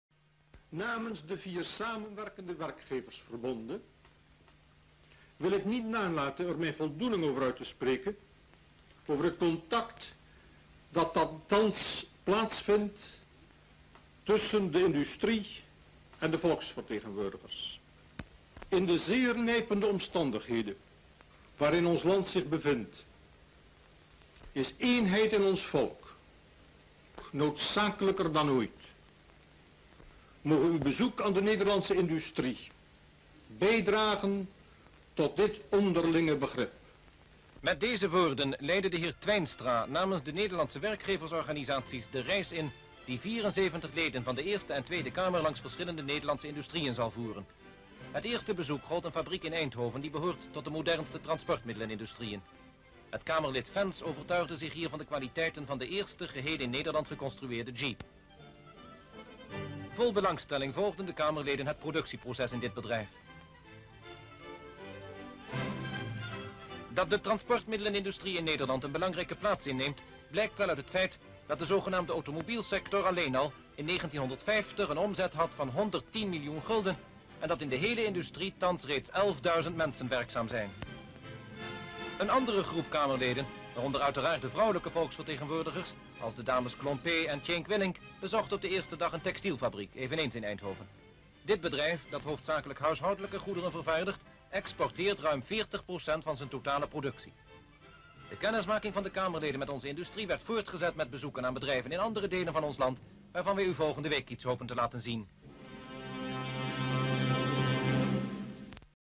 Members of the Senate and the House of Representatives pay a visit to the Dutch industries (Wee 🔴
Description (more): Representatives from the Dutch Parliament visit the lorry factory DAF and a textile factory in Eindhoven. SHOTS: - Mr. T.J. Twijnstra gives a word of welcome on behalf of the Dutch employers; - DAF: the men tour the factory grounds and look at several car chassis' made in the factory; they walk through the factory halls and watch how the workers work with a lathe and in an assembly hall they assemble a lorry in a production line; - int. textile factory: another group of representatives walks through the spinning mill and watches how the thread and fabrics are produced; 00:49 J.A. Jonkman (three-piece costume); 00:51 J.J. Fens makes a test drive in a jeep; 01:26 M. Klompé; 01:29 M. Tjeenk Willink.